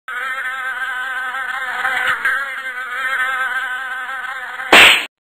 Smashed Fly
Pencil test of a smashed fly